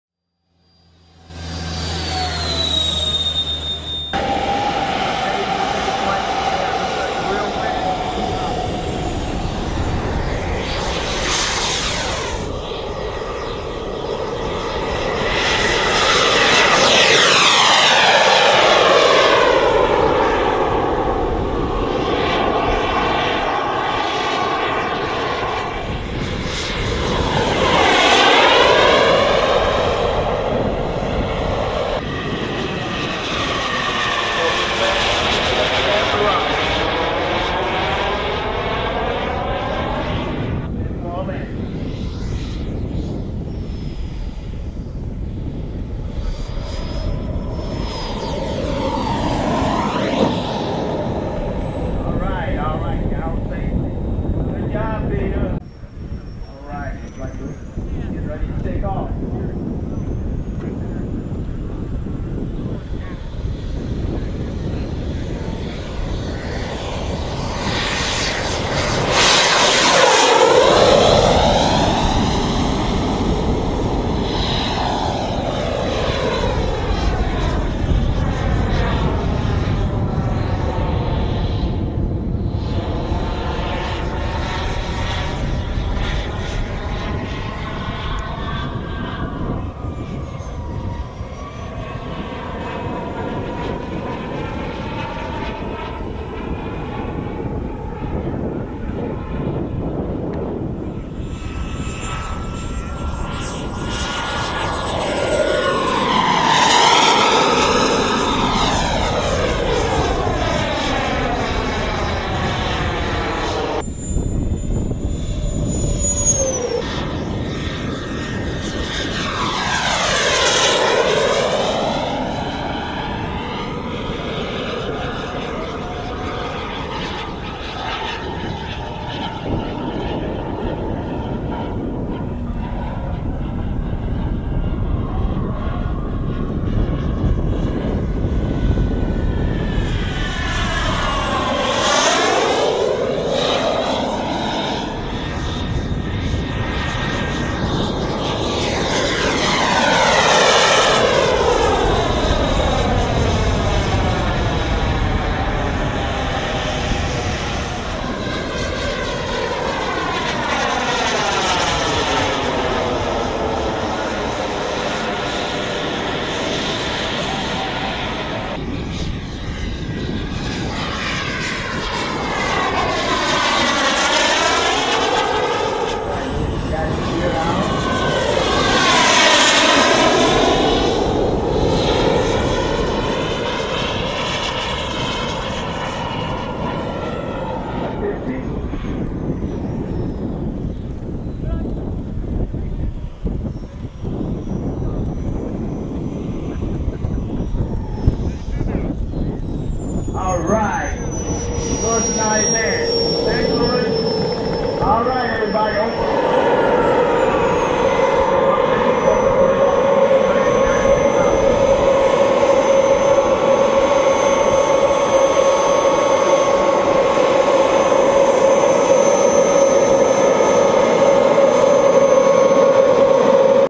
Michigan_Jets_August_2010_08
final clip we shot of the days flight videos from Michigan Jets RC event, great pilots, planes of course and a lot of fun as usual..